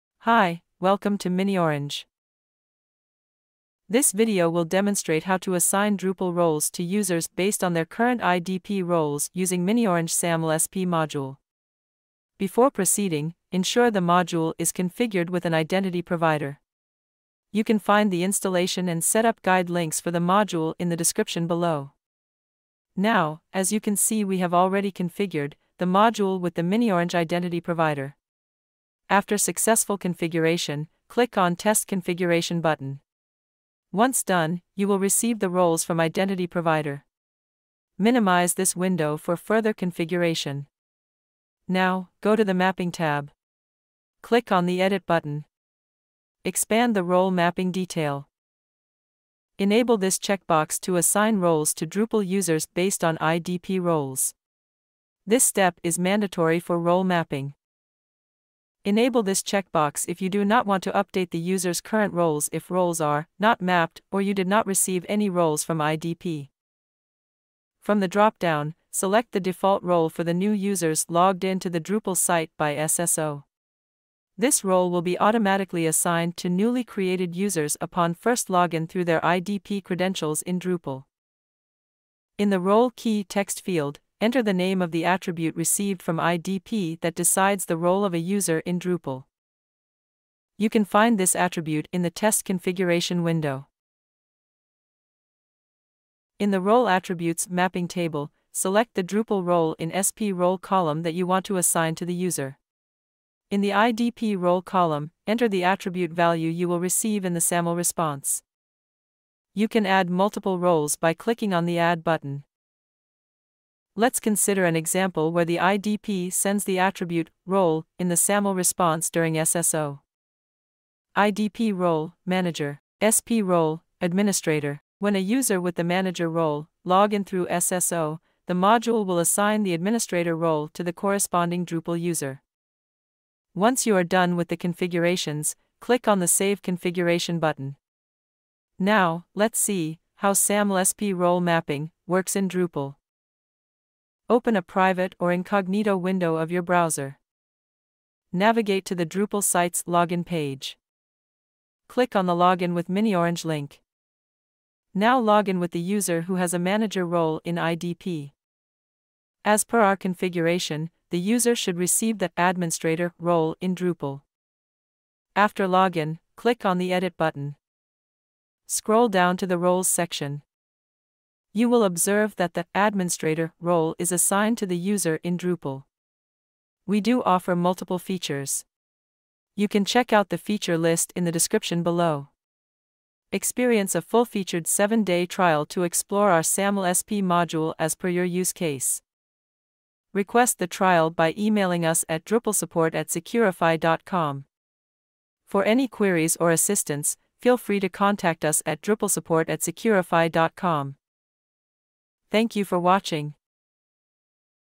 Drupal Single Sign On - Role Mapping | Drupal SAML SSO Module
This video will show you how to map IdP user roles to the Drupal user roles of the corresponding user. In this video, we are using the miniOrange SAML SP module which allows seamless SSO between Drupal and any IDP that supports authentication using SAML protocol.

Feature List:
1)Role mapping
2)Disable auto-creation of users in Drupal
3)Protect Your Website
4)Auto redirect user to IDP
5)Backdoor Login
6)Domain Restriction
7)Import Export Configurations

Timestamps:
0:03 -  Introduction
0:13 - Download and Installation of Module
0:24 -  Module Configuration
0:45 -  Role Mapping
2:21 - How Role Mapping works
2:57 - Feature List